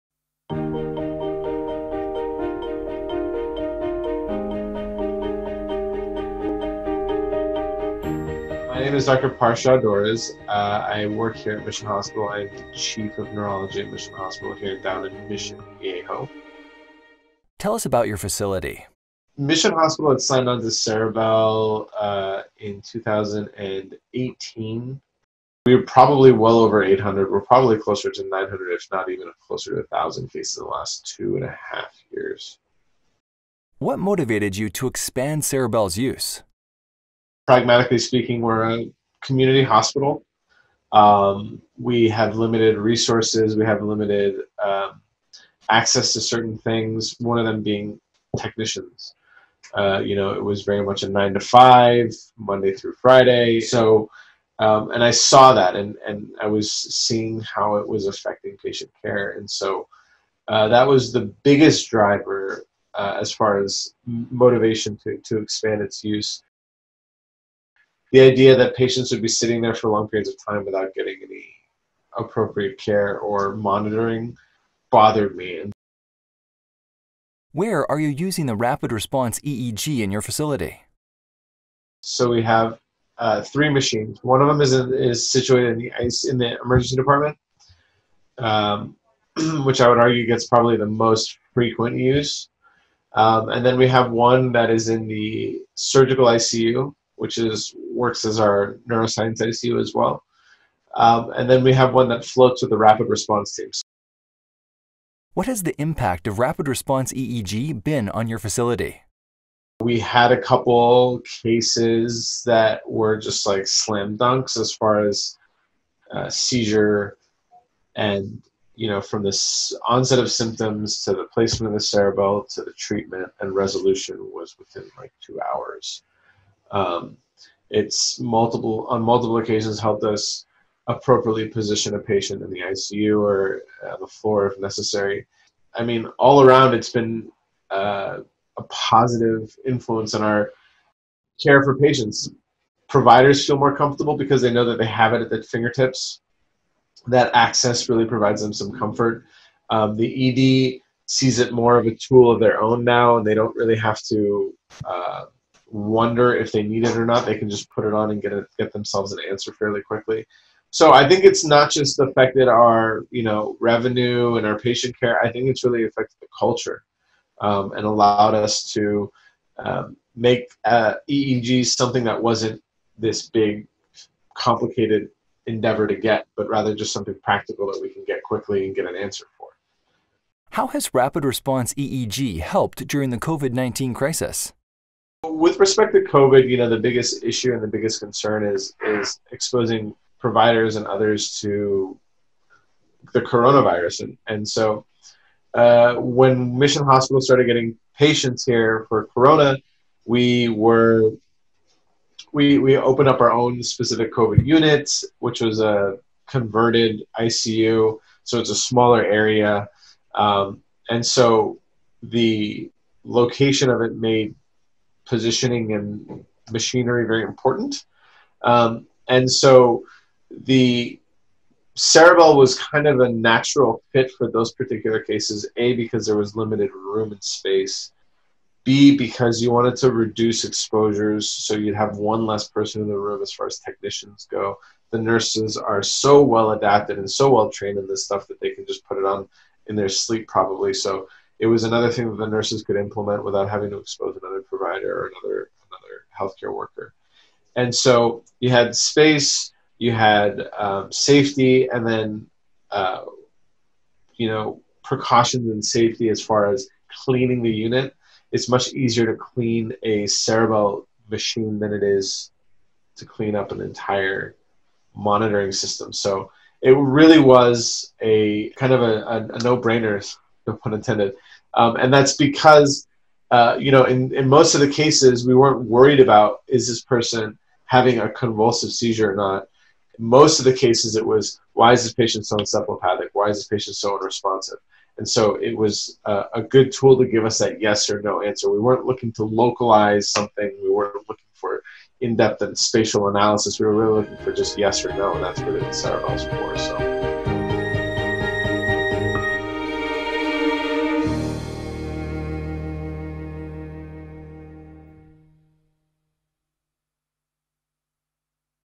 Dr. Parshaw Dorriz shares real-world experiences: Impact of Ceribell Rapid Response EEG
Dr. Parshaw Dorriz is the Head of Neurology at Mission Hospital in Mission Viejo, California.